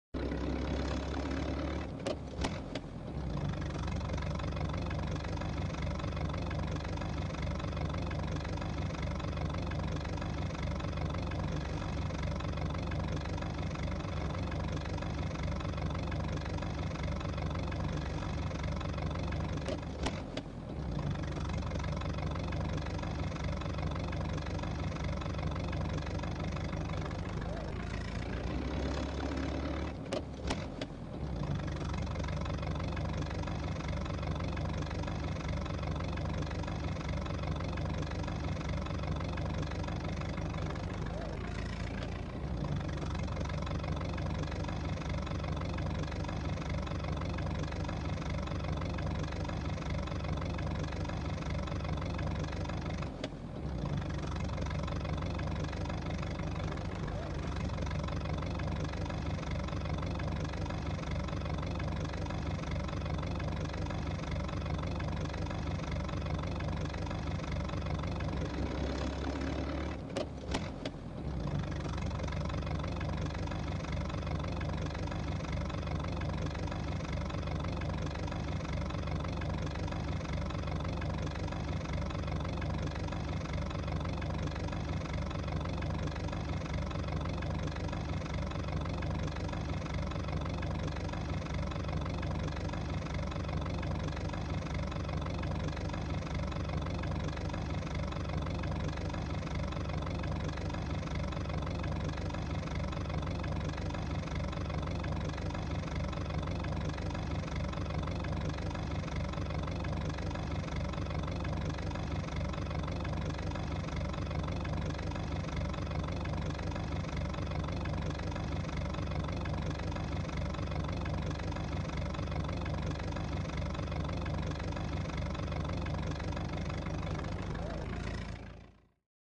VINTAGE CARS ON STREET IN 1930'S
The view of 1930 street of beverly hills .Very beutyfull view  of the road on that time .The vintage cars rocks on road .